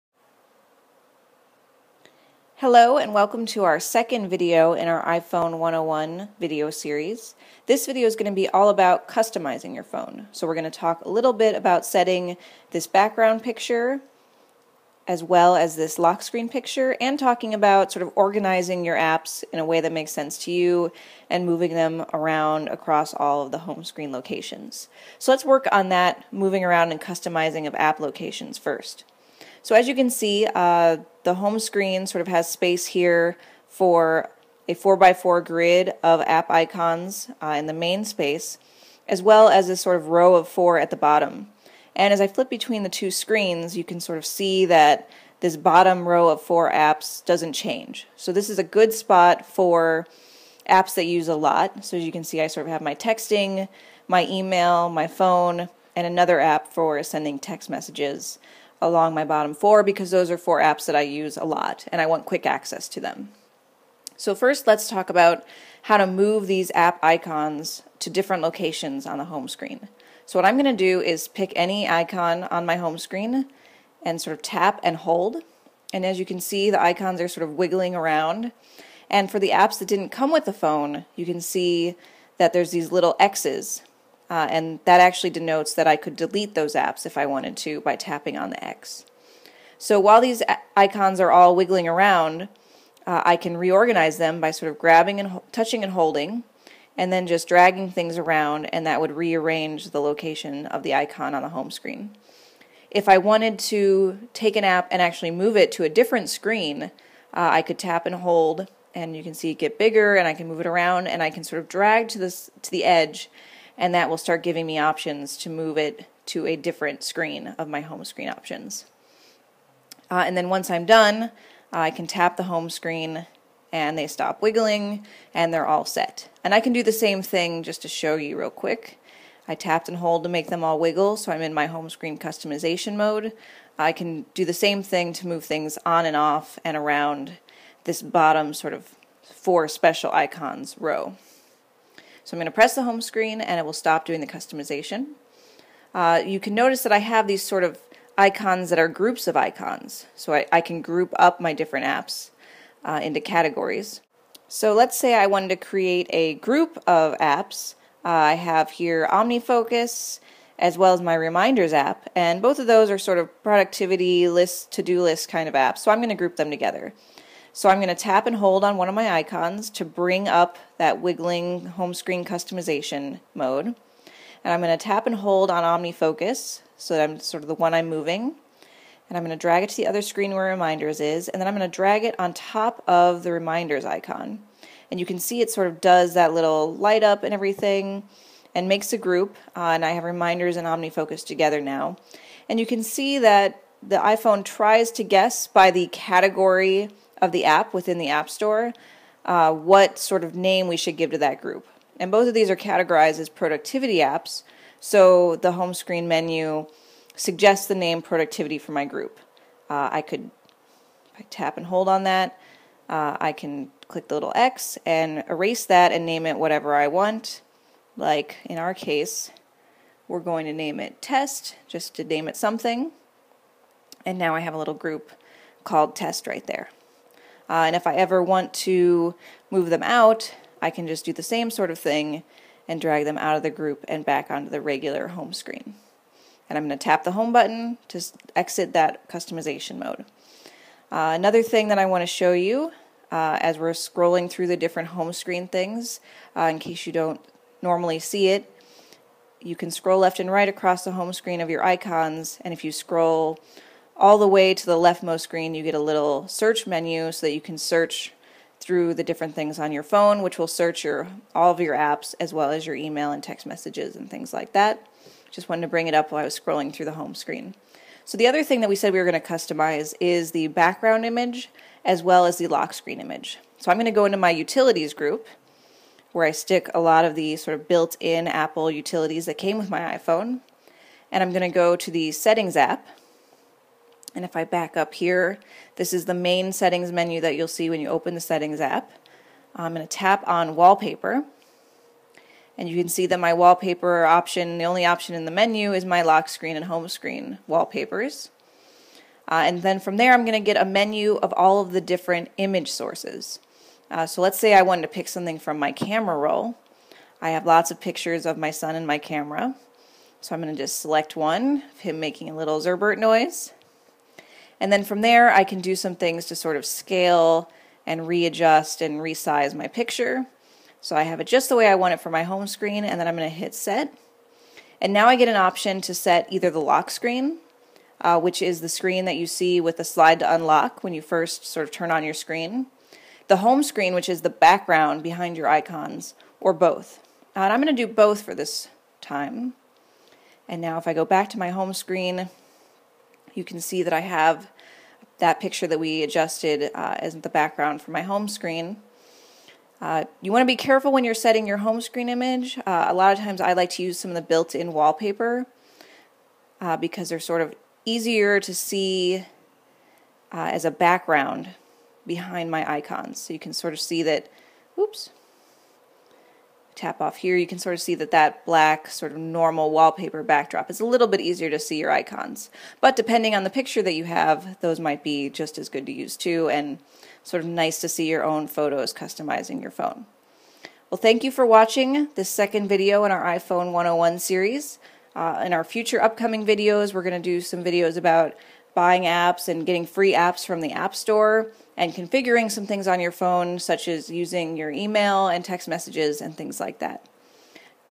iPhone 101 Volume 2. Customizing your iPhone
Organizing app icons on your home screen, as well as changing the images on your home and lock screens.